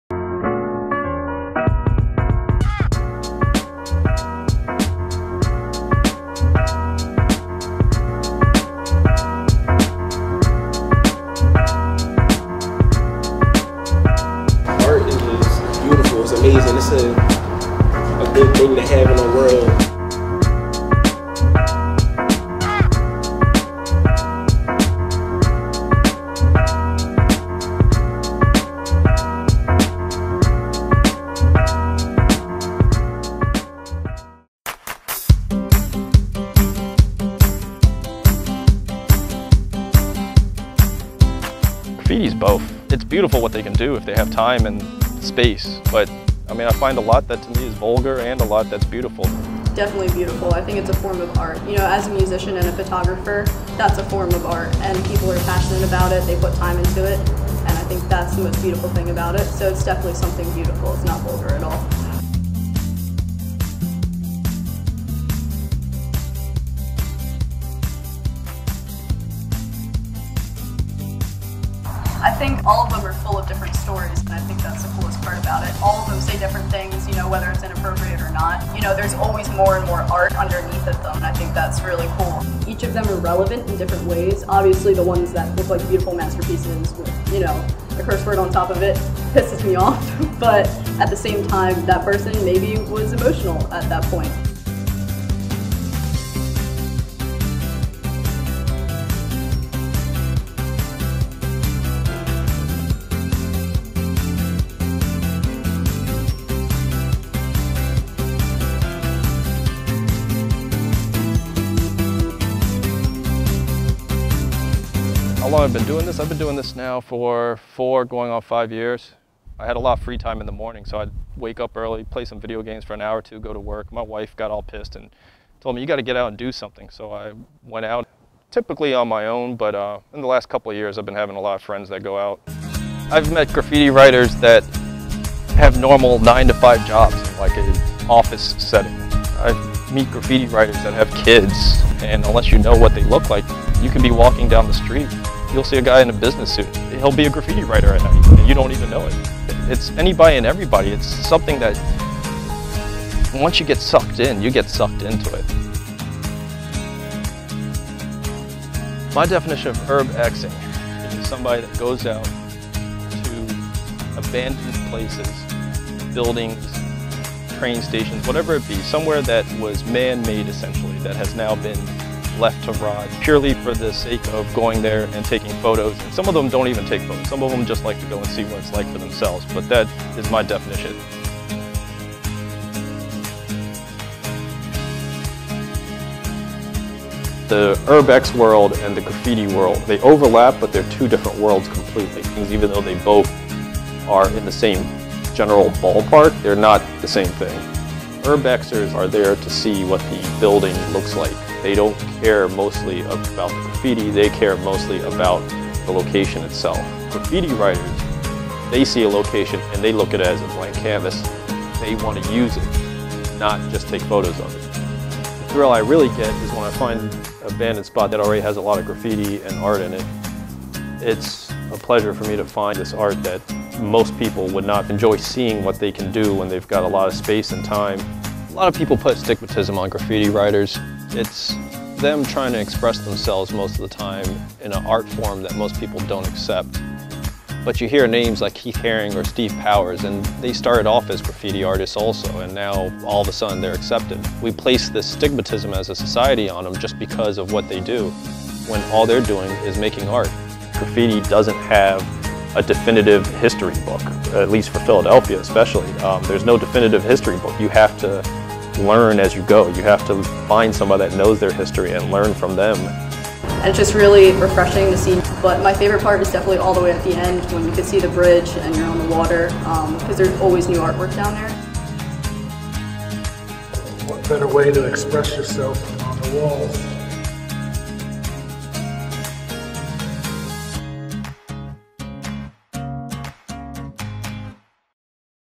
Graffiti Documentary RCBC
Graffiti Documentary for TV Production Class - made by Sarah, Katie, Anthony, Zeid & AnnaRose